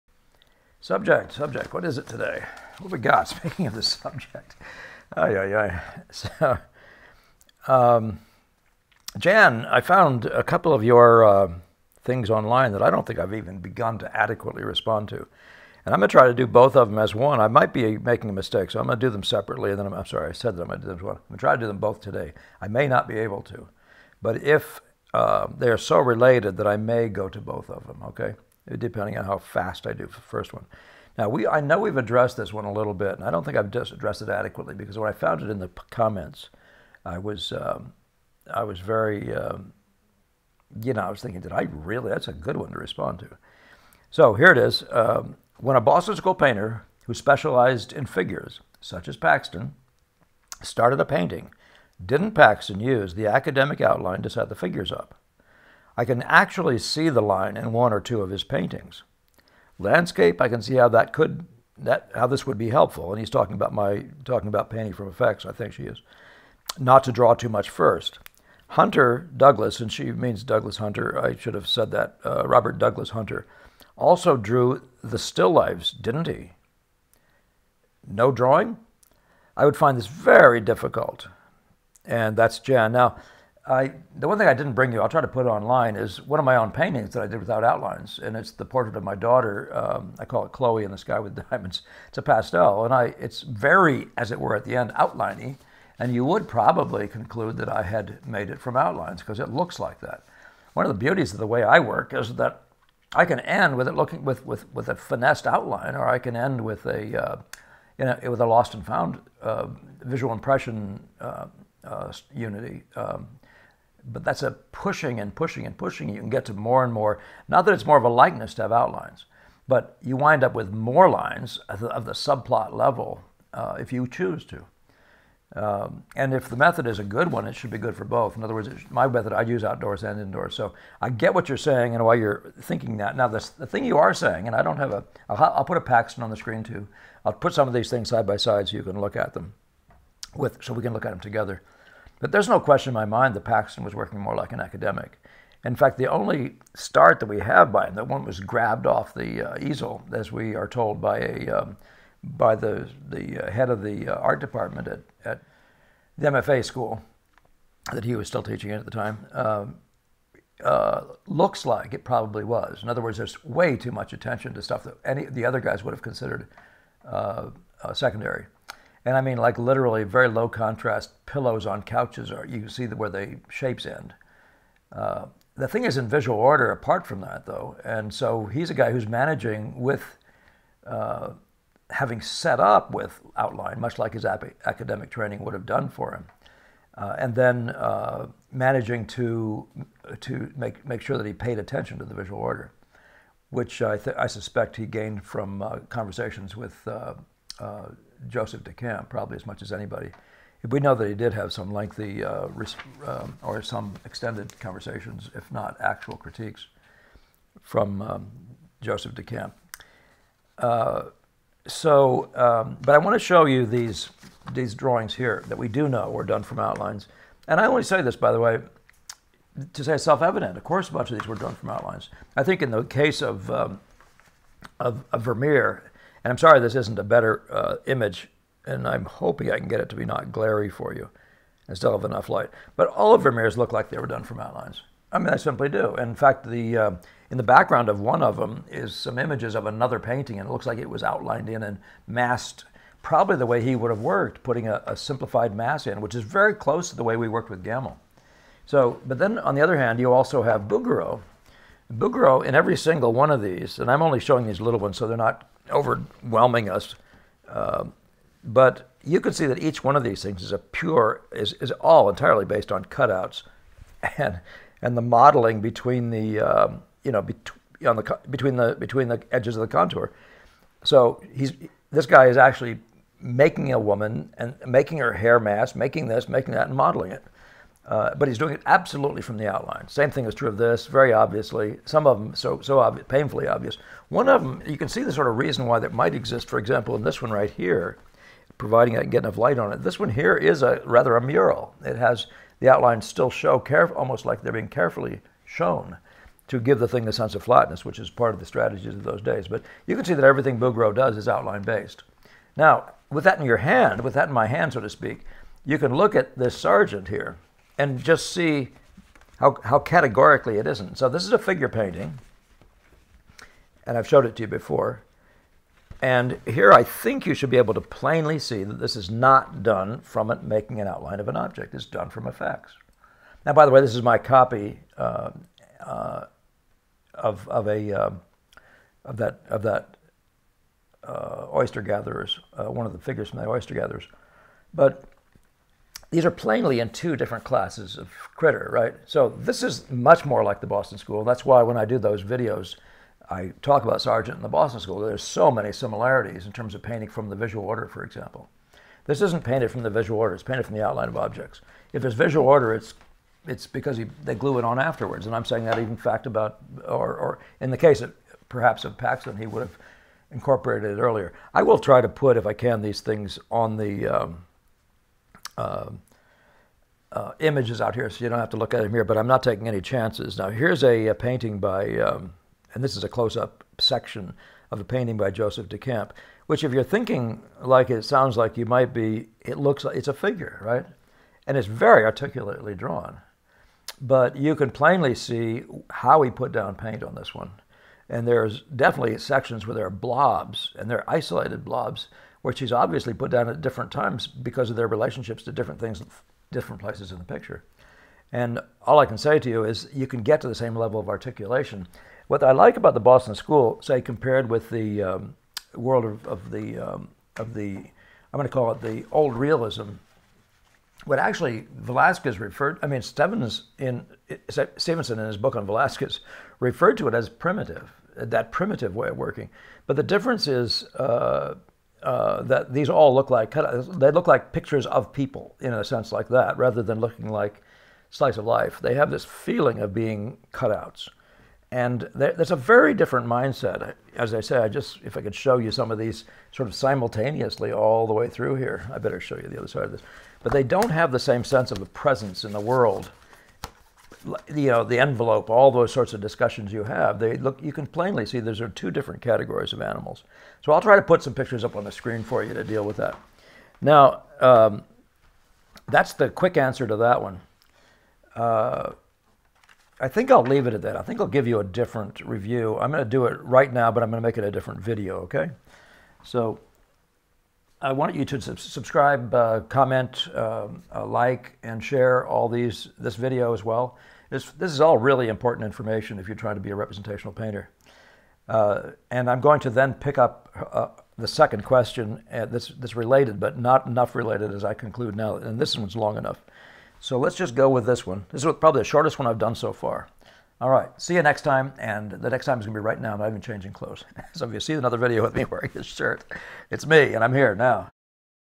Paul Ingbretson Talks about Preparatory Outlines and Alternatives - No. 52
Outlines in the lay-in of a painting and their limits in Boston School Impressionism

In response to Jan